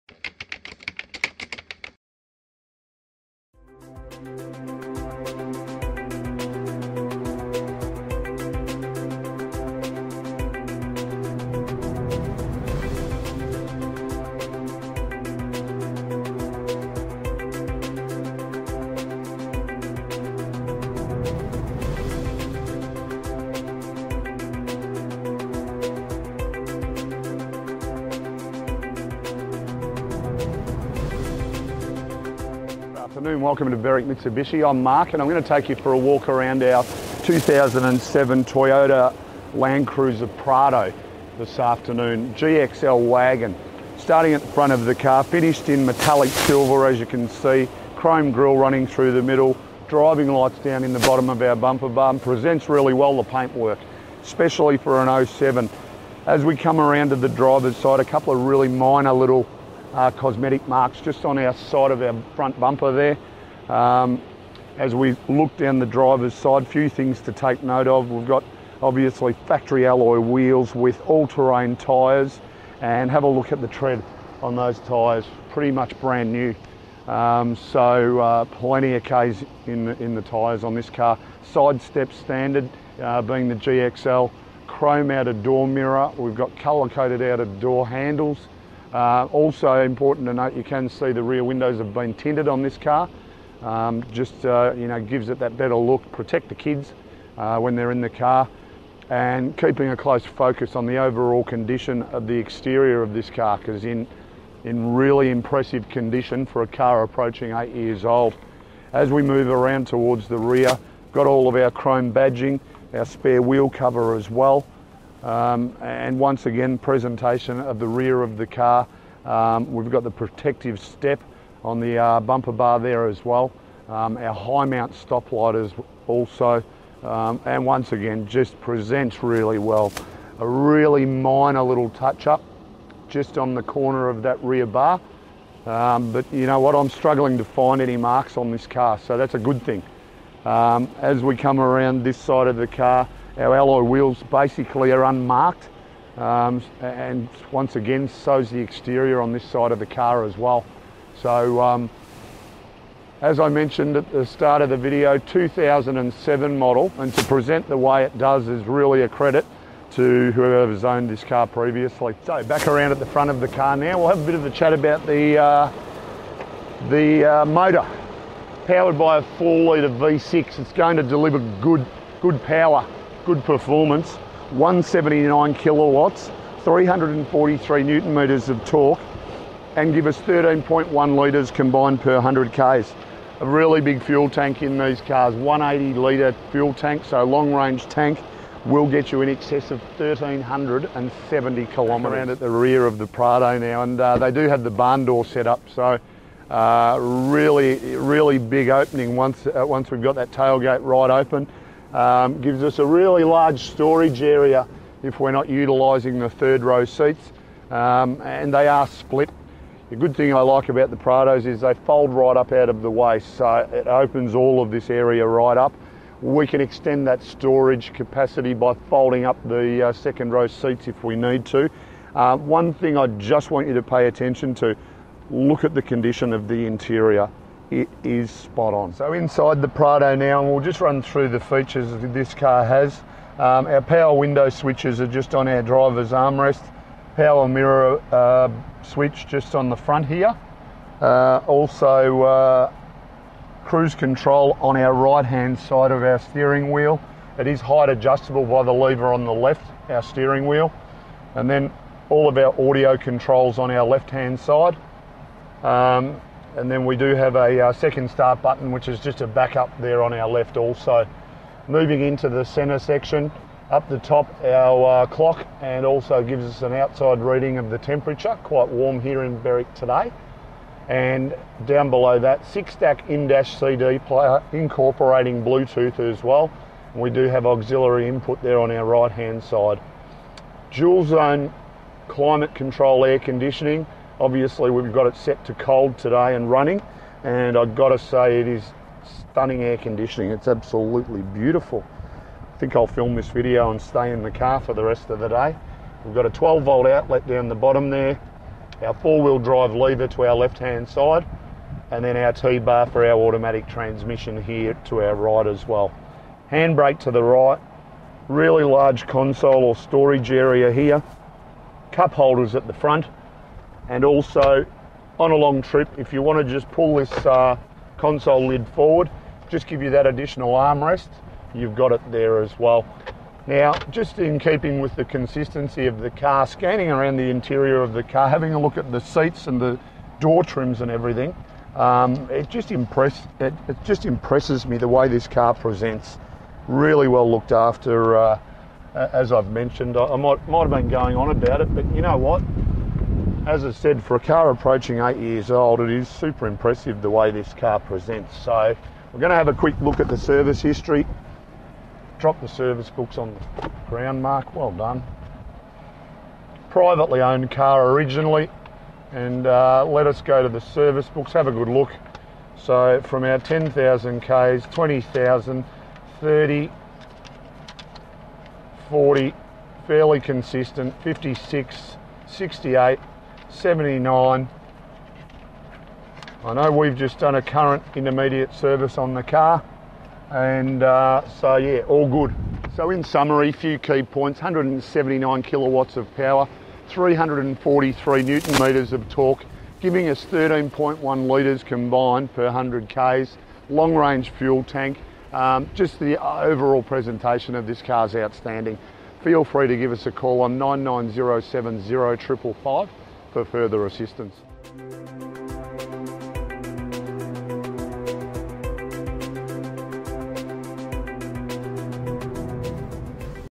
2007 Toyota Landcruiser Prado GXL Auto 4x4 Review - B4872
A video walkaround tour of the 2007 Toyota Landcruiser Prado GXL Auto 4x4 Review - B4872 Presented by Mark Mcgregor Used Car Sales at Berwick Mitsubishi,